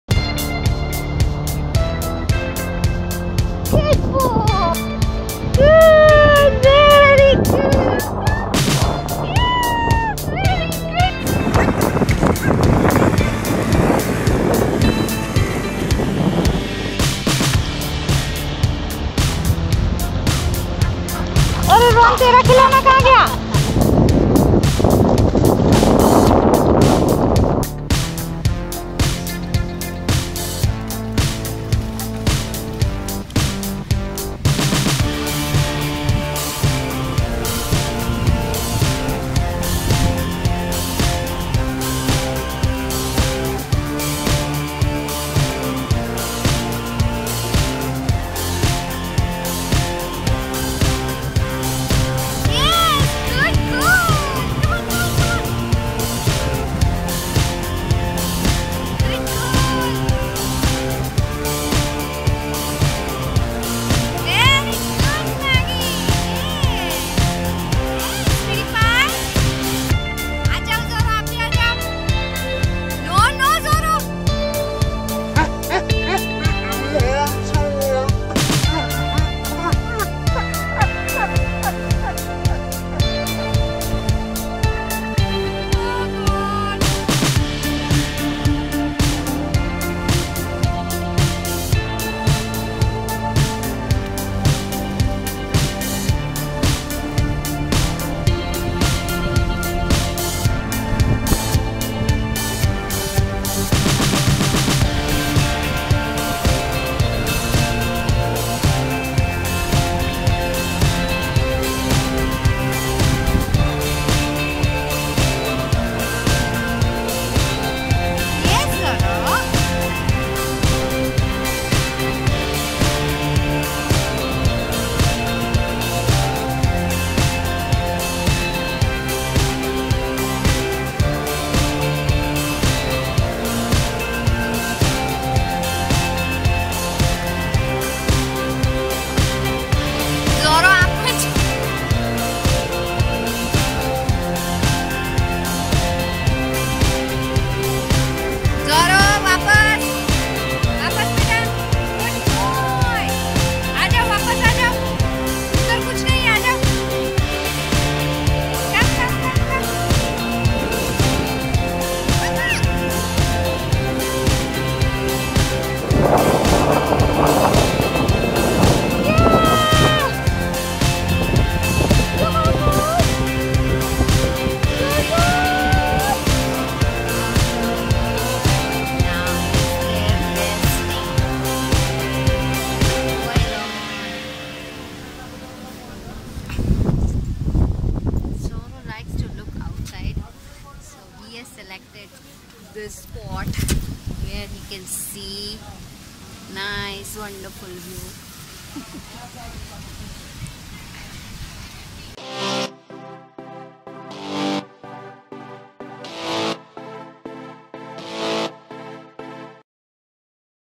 Life of 4 dogs | Unlock 2 story | Pet Beach Picnic | Mumbai India 2020
Today we'll be taking you all on a pet beach picnic.

Mental and physical health fullfill of all 4 dogs after a very long COVID-19 lockdown.

Featuring the Dogs

Maggie (Labrador retriever) 10yrs old. Petparent Mr. Piyush and Mrs. Kunjan 😊 that's us.

Zuzu (Lhasa apso) 7 and half yrs old. Petparent Mr. Piyush and Mrs. Kunjan 😊 that's us.

Zorro (Labrador retriever) 7yrs old. Petparent Mr. Pratik and Mrs. Aaradhna

Ron (Golden retriever) 3yrs old. Pet-parent Mr. Jaywant Ghodke and Mrs. Shilpa Ghodke.